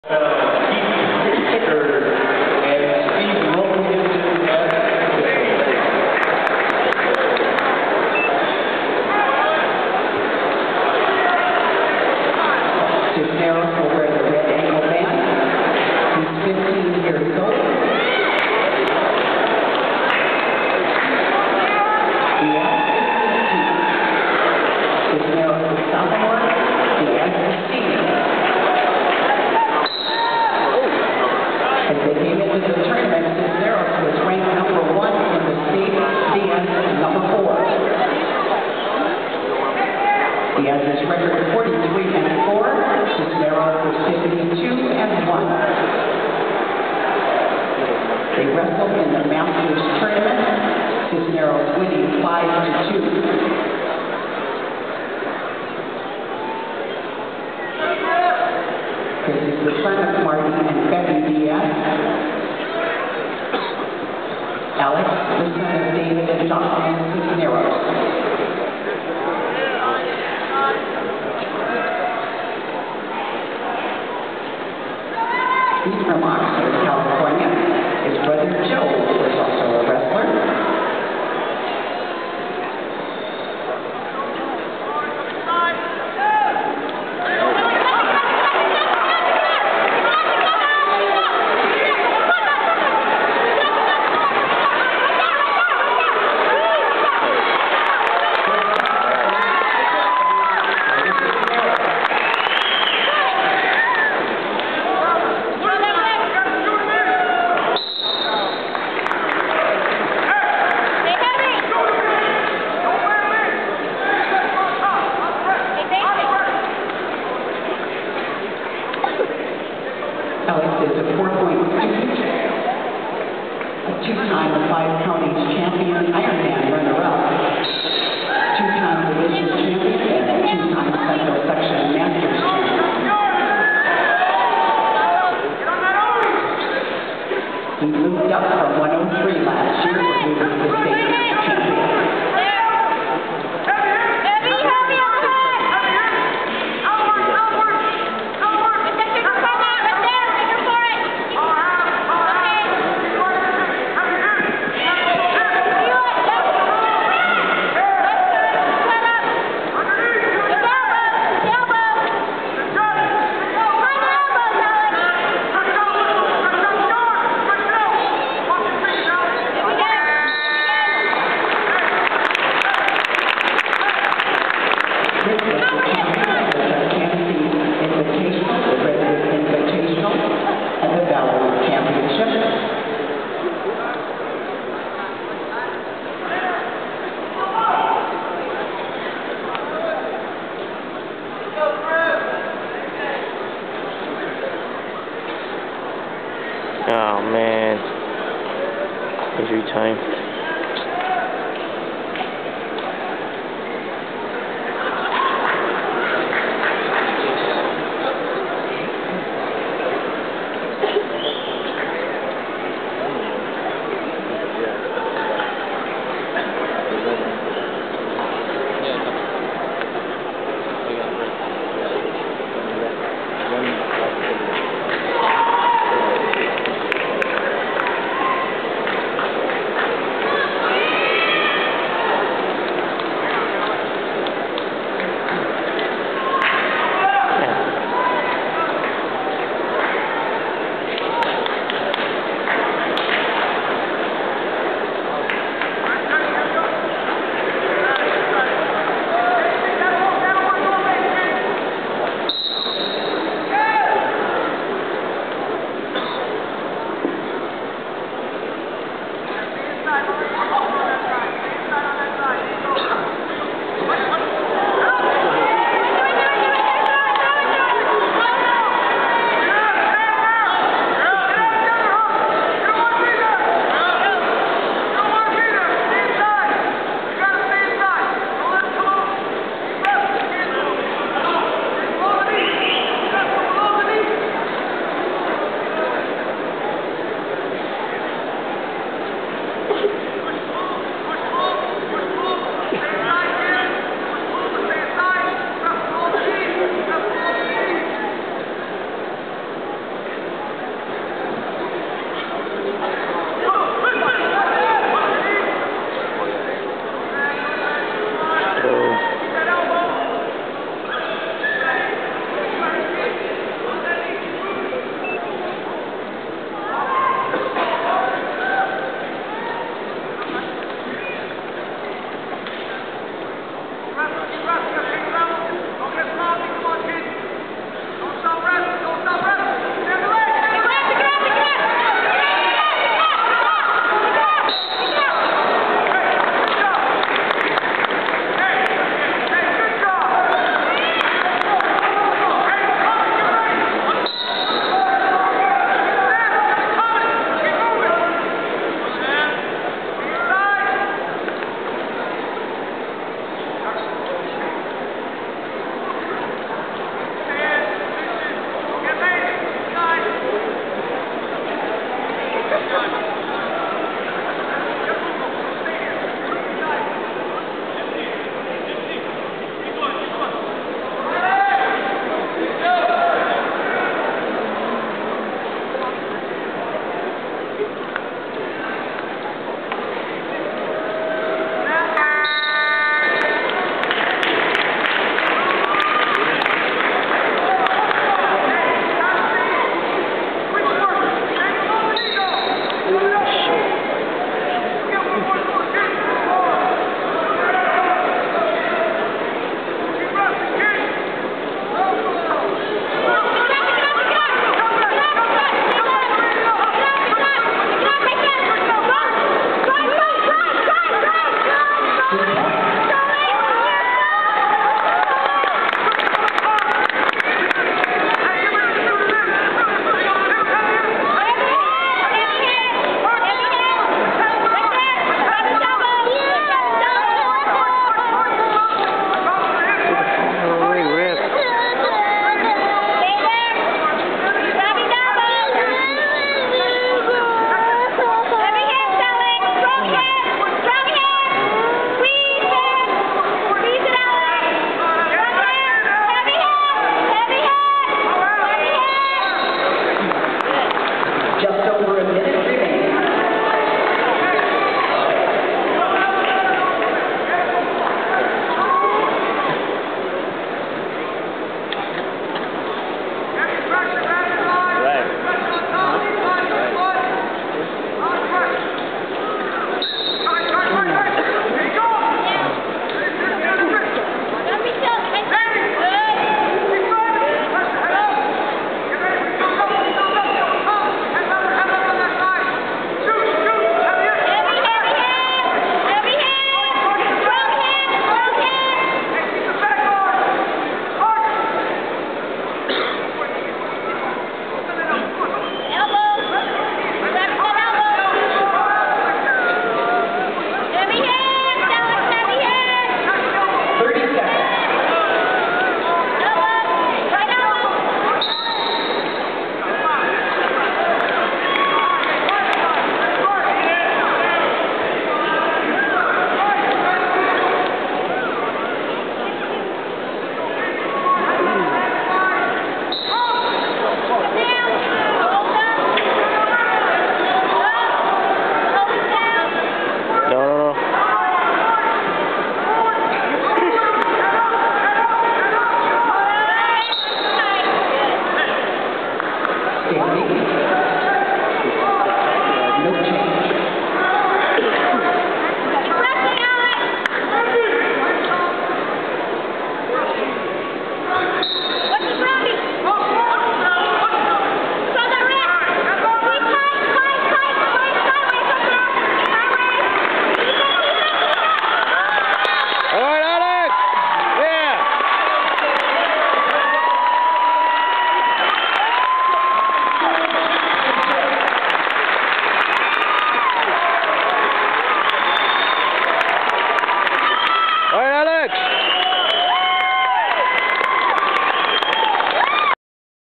2010 C.I.F 112LB state final Alex Cisneros(Selma)CA vs Chris Diaz(Sanger)CA
112lb final for california state championship, Cisneros won 5-0.